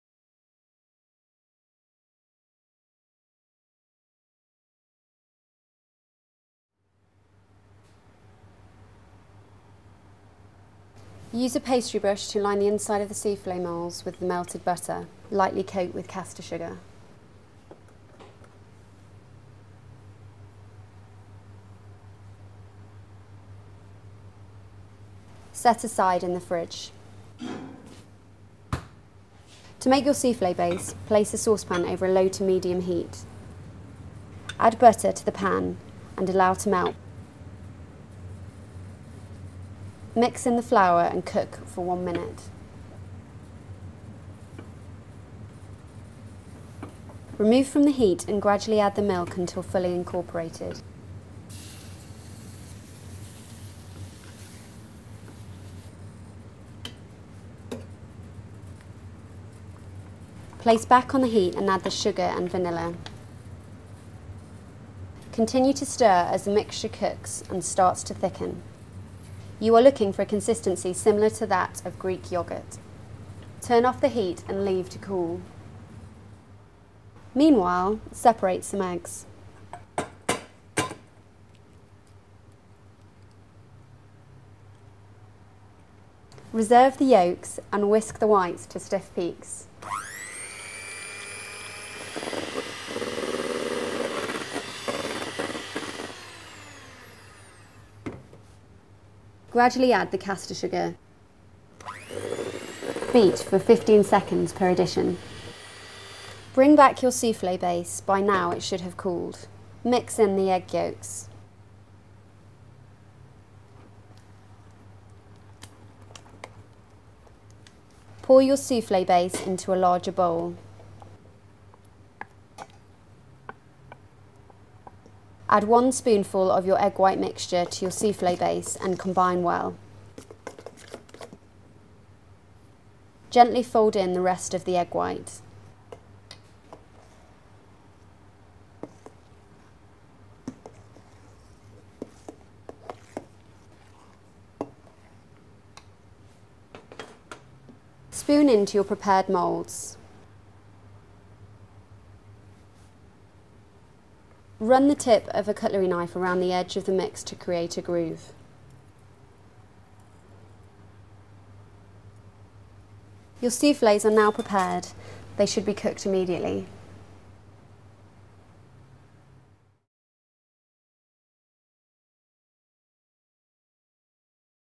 How to make a sweet soufflé
Great British Chefs demonstrate how to make a sweet souffle in this easy to follow video.

For more tips and videos, along with recipes from the UK's leading chefs,